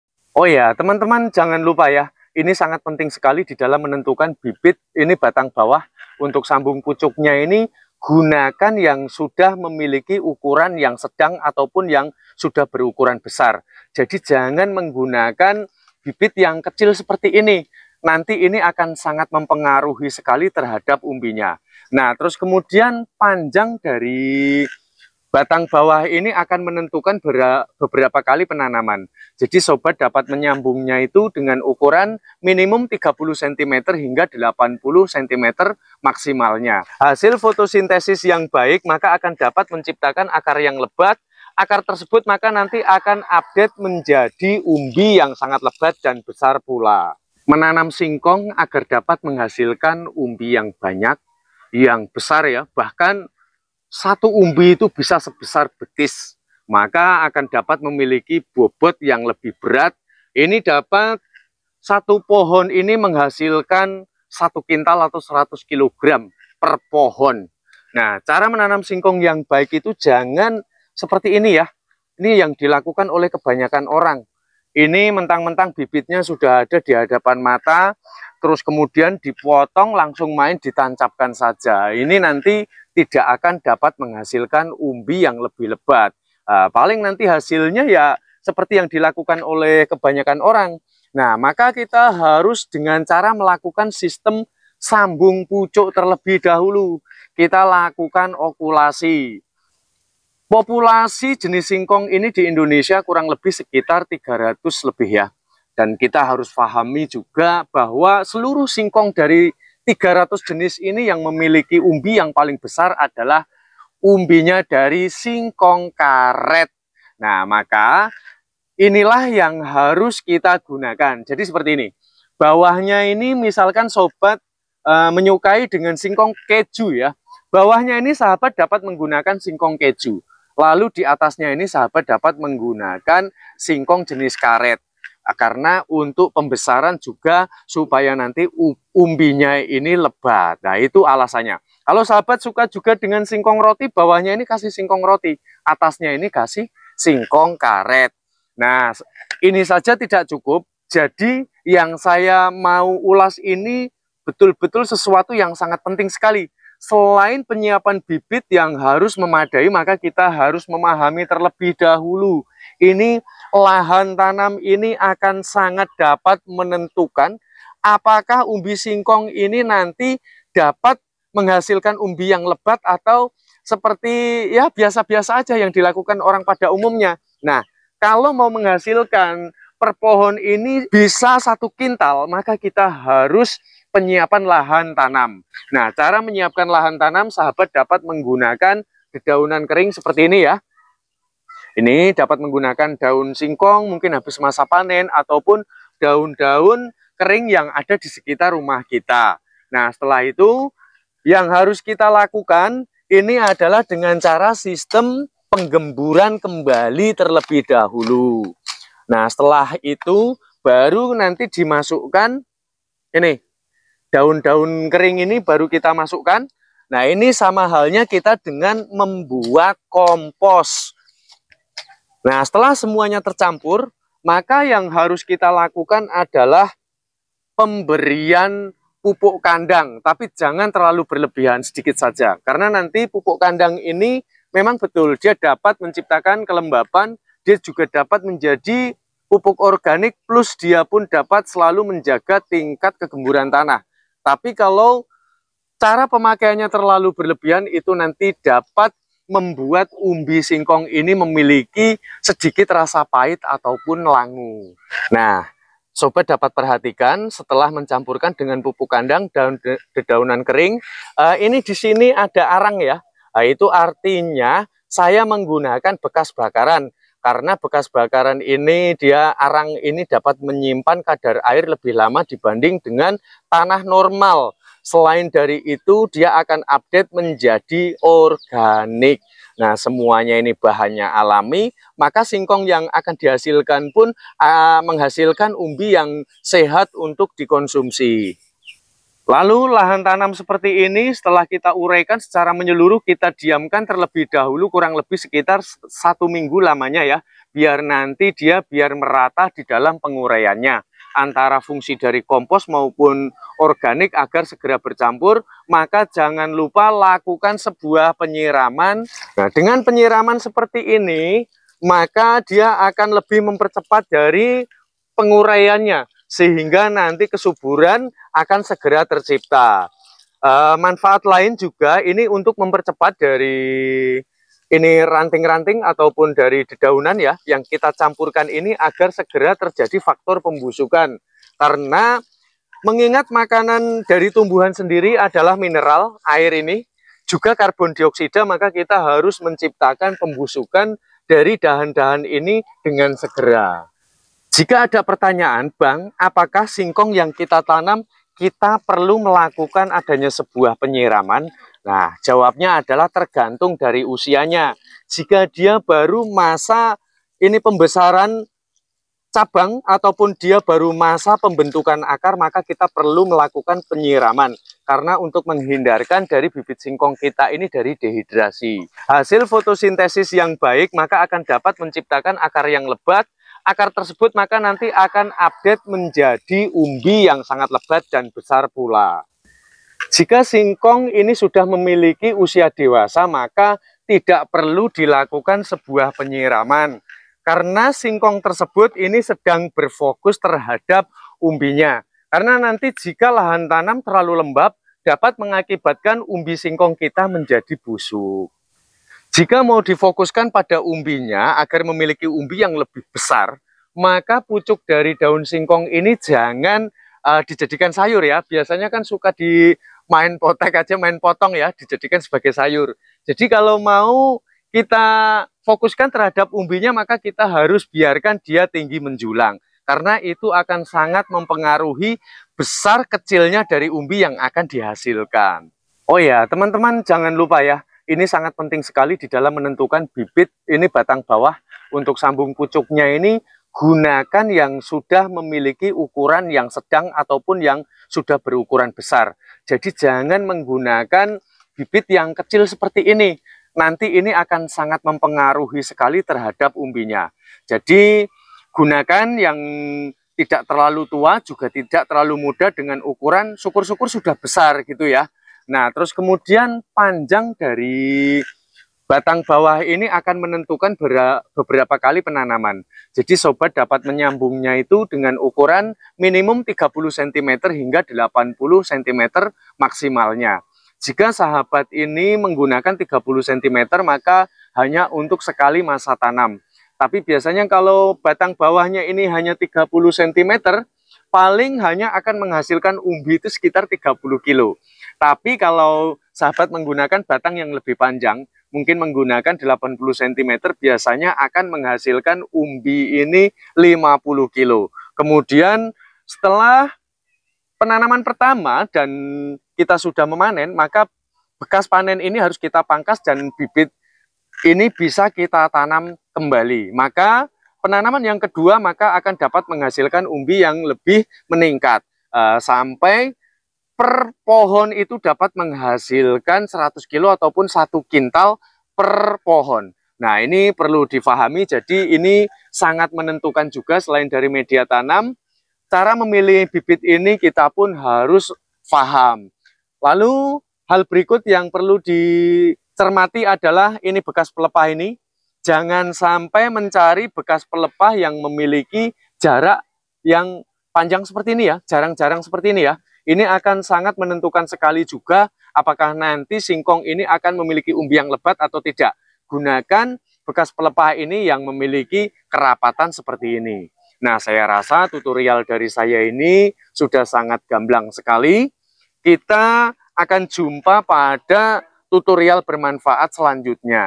tanam okulasi singkong agar umbinya 50 sampai satu kuintal per pohon
Teknik tanam singkong sistem okulasi agar berbuah banyak dan besar menghasilkan hingga 50 sampai satu kuintal per pohon